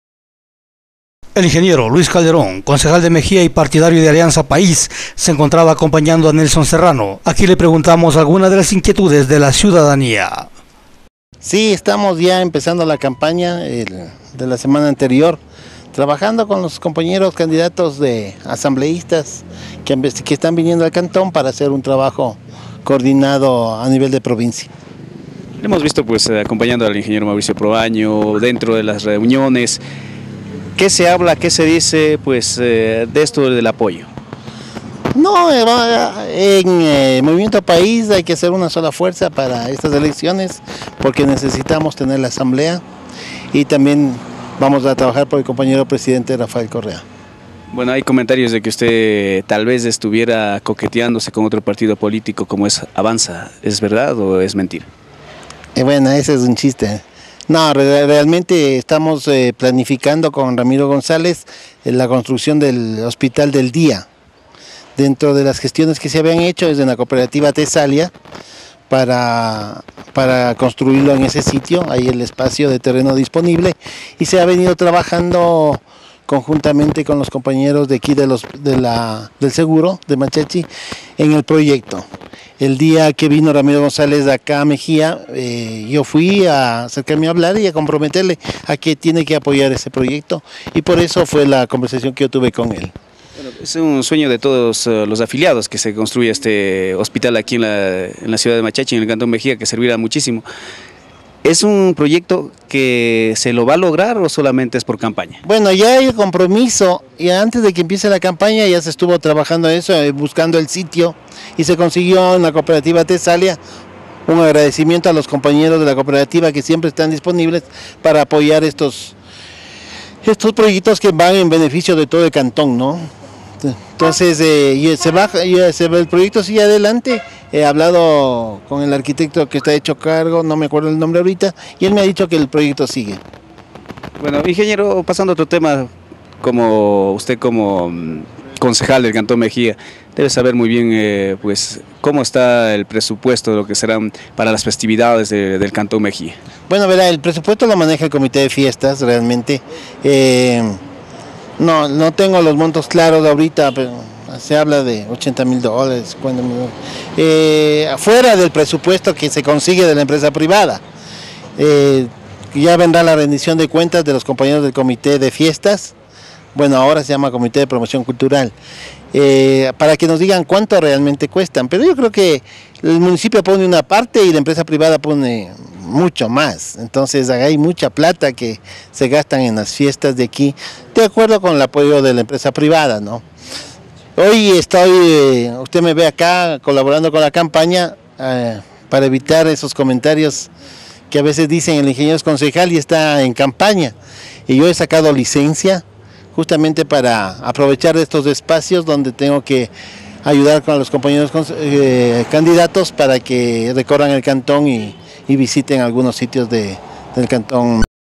EL ING. LUIS CALDERON ACLARA CIERTOS RUMORES
EL ING. LUIS CALDERÓN CONCEJAL DE MEJIA Y PARTIDARIO DE ALIANZA PAIS SE ENCONTRABA ACOMPAÑANDO A NELSON SERRANO EN SU RECORRIDO DE MEJIA, AQUÍ LE PREGUNTAMOS  ALGUNAS DE LAS INQUIETUDES DE LA CIUDADANÍA.